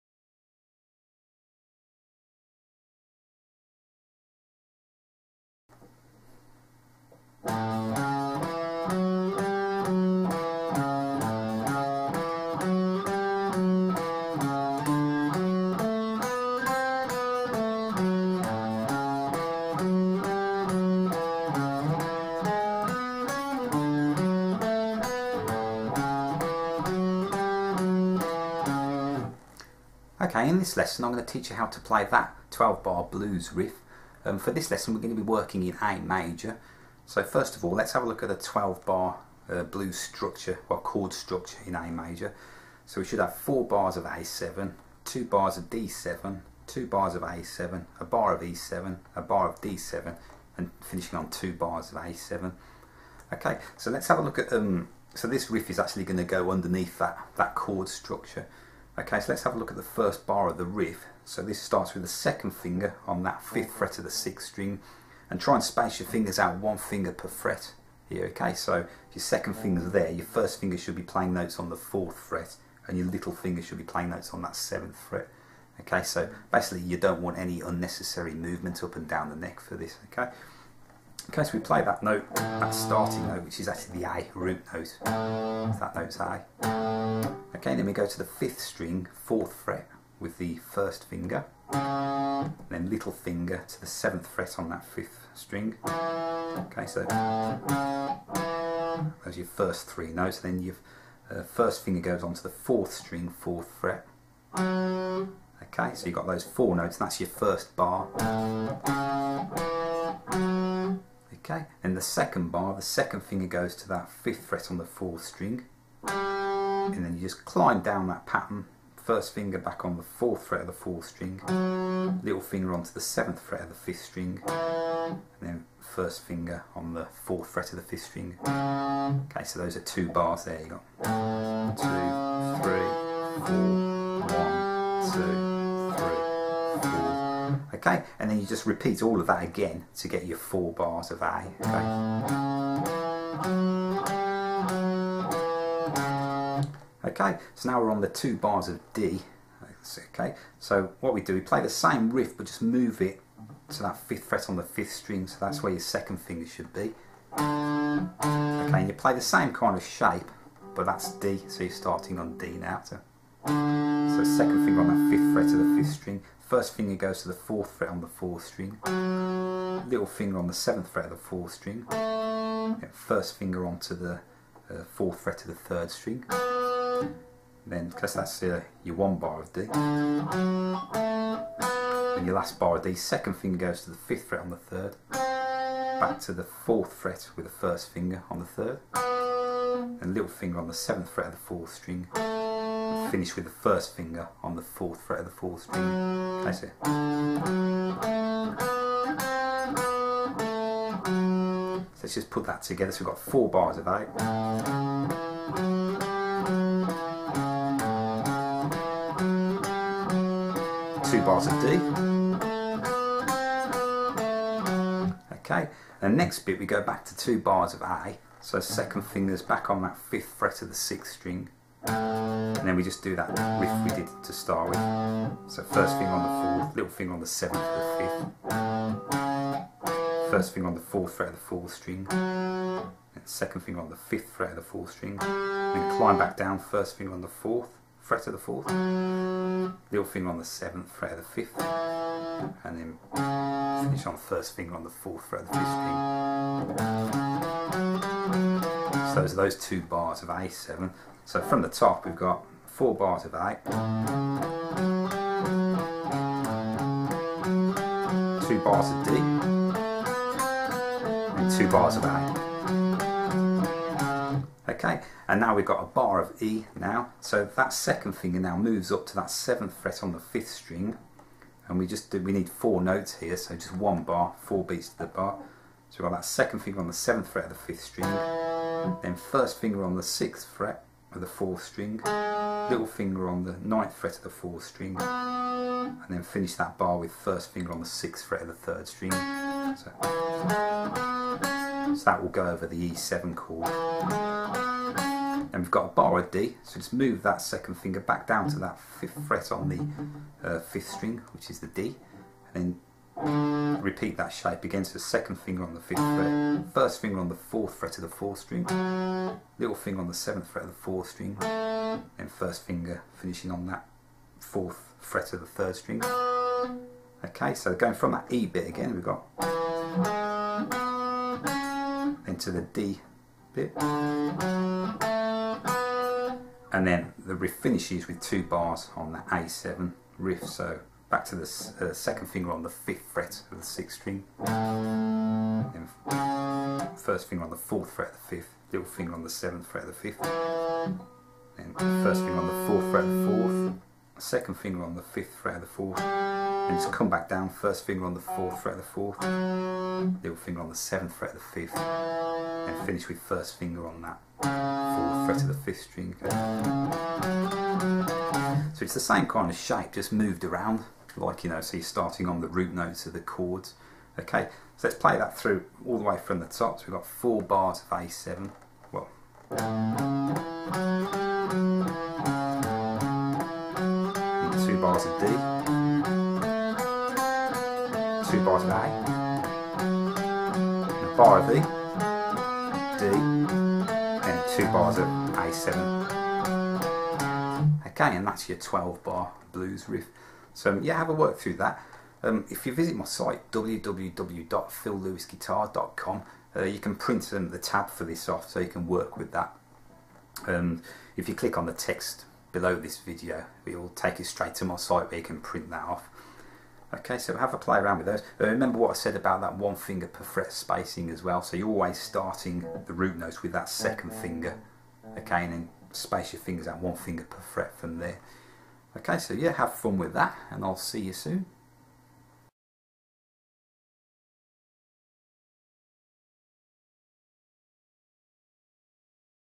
Simple 12 Bar Blues Riff On Guitar
A guitar lesson showing you how to play a basic 12 bar blues riff.  The example in the video is given in A major.